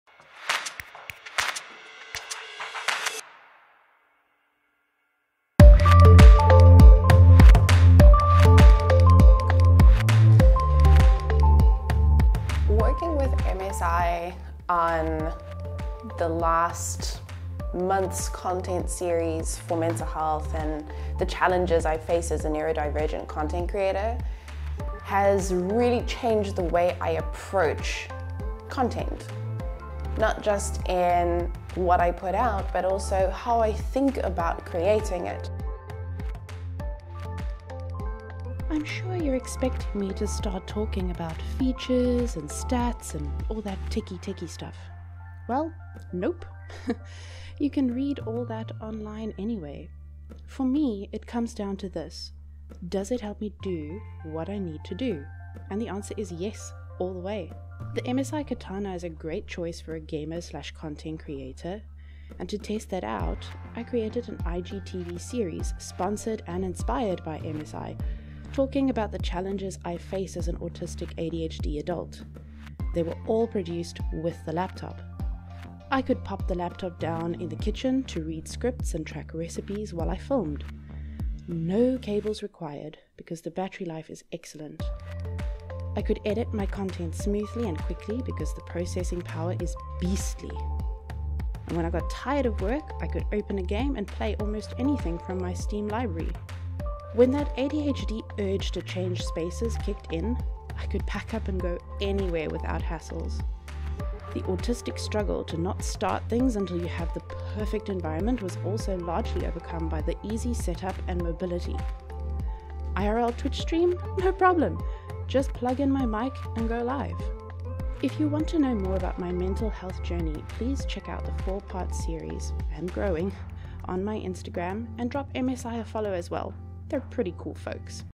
Ruthy0nfire x MSI Katana
I teamed up with MSI to share a little about my life as an autistic/ADHD content creator. For this project, I got to use the MSI Katana gaming laptop and this is my experience in a nutshell!

Check out the technical specs below and drop a follow on the socials. I'd love to hear from you!

Thumbnail: Moosli Bowl